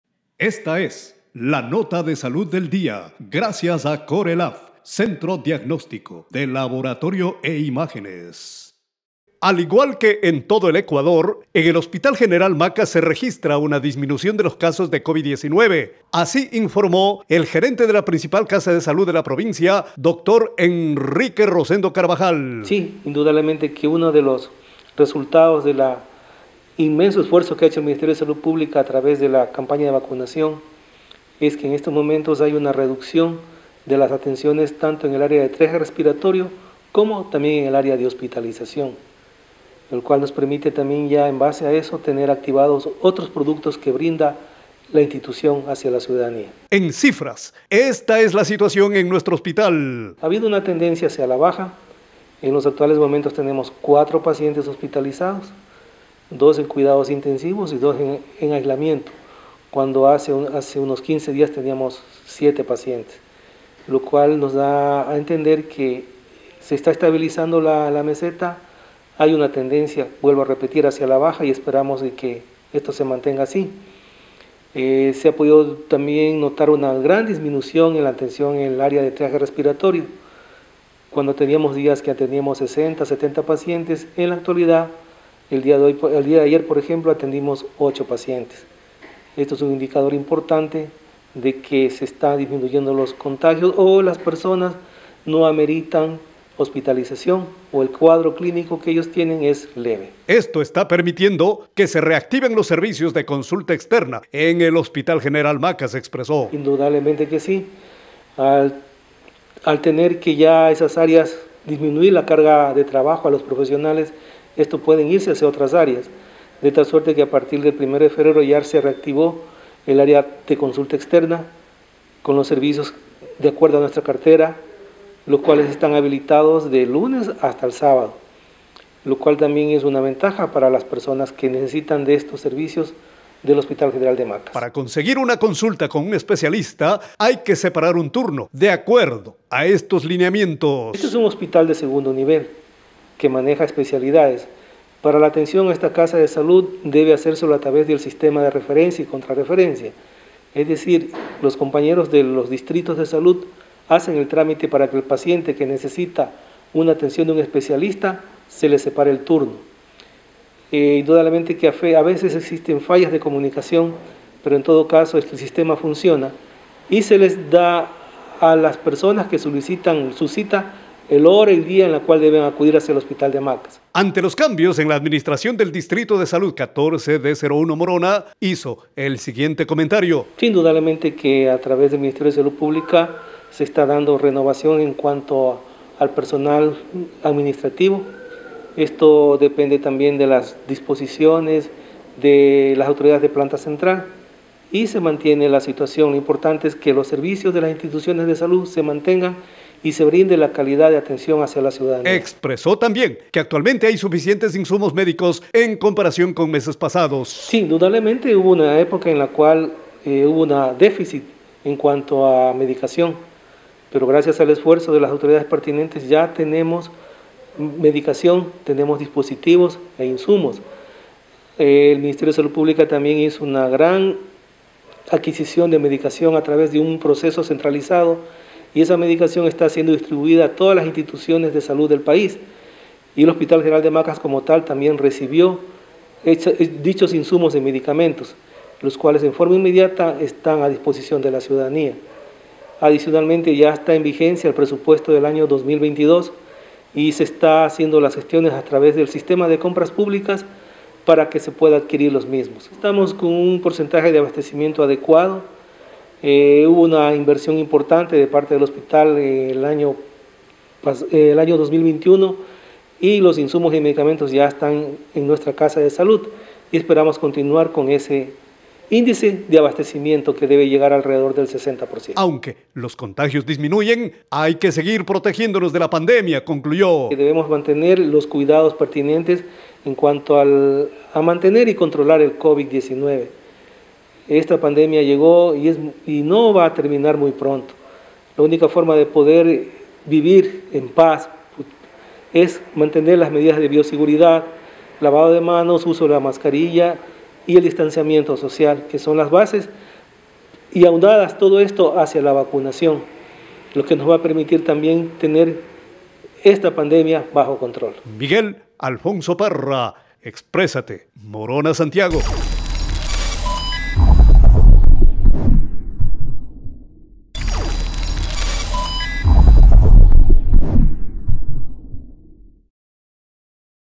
Disminuyen casos de pacientes internados por Covid 19 en el Hospital General de Macas.
[SALUD] ATENCIÓN: MoronaSantiago, disminuyen contagios de covid 19 y pacientes internados en el Hospital General de Macas. El Dr. Enrique Carvajal, gerente de la entidad, informó las cifras actuales.